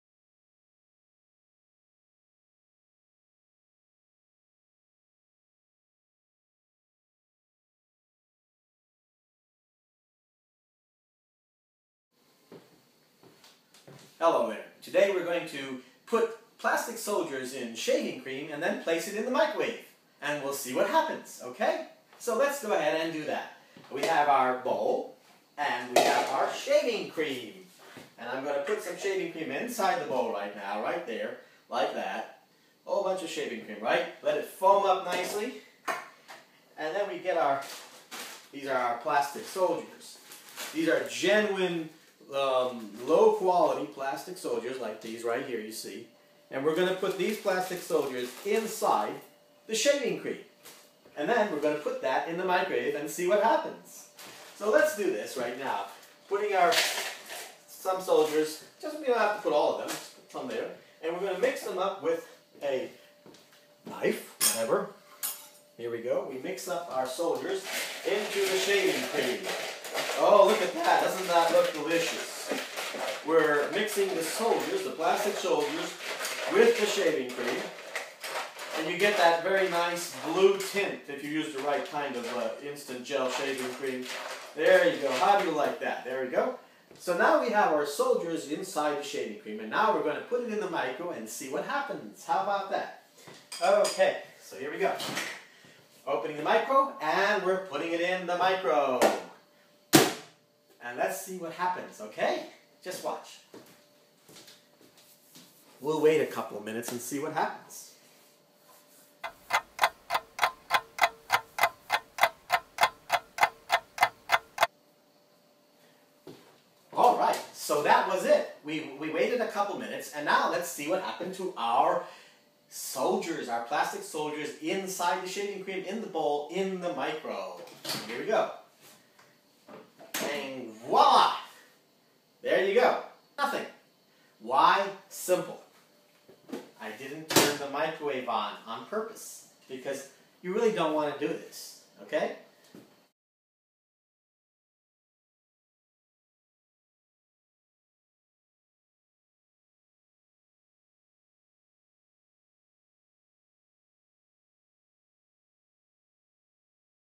Microwave Magic! Toy Soldier, Shaving Cream!
This is For Comedic Entertainment Purposes Only! Do not try this at home!
Made using iPhone, olloclip and iMac ( iMovie Editor ). Enjoy!